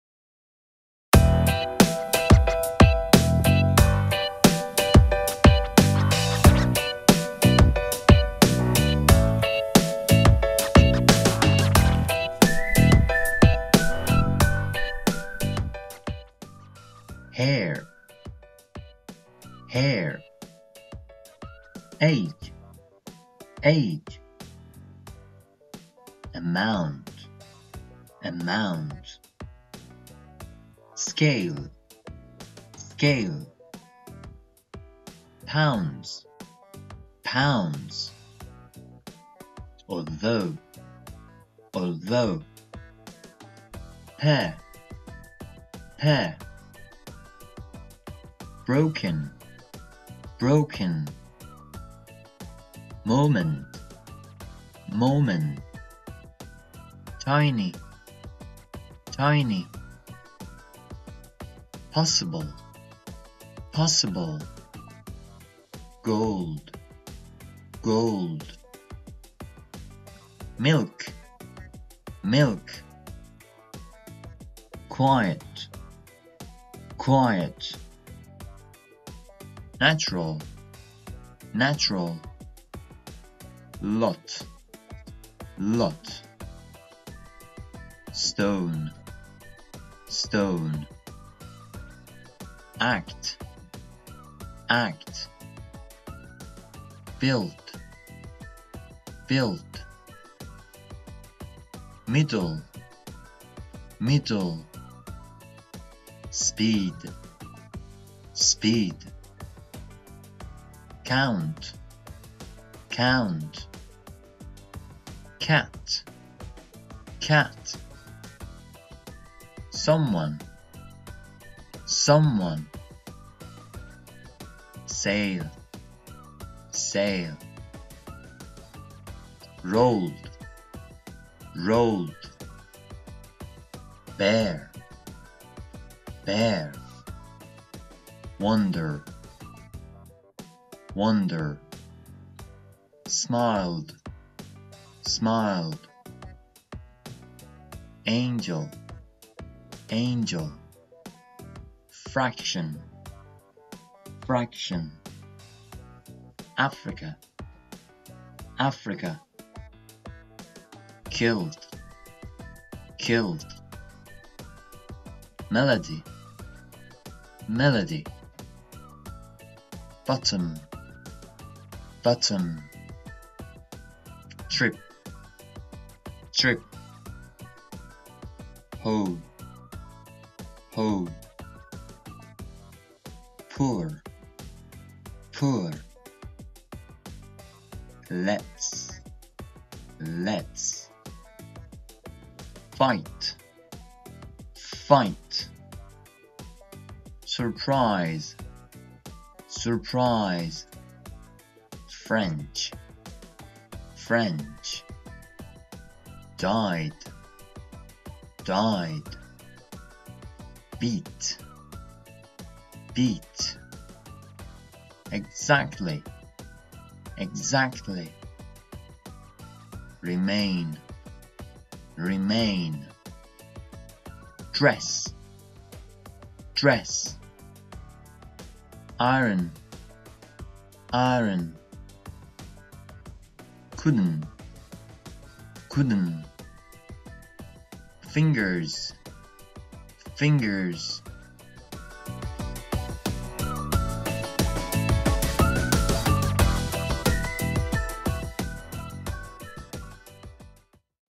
1000 common English words 651-700
In these 20 videos you have 1000 most commonly used English words written and pronounced.
درس ۵۰۹
Lesson 509

Skype: english4persians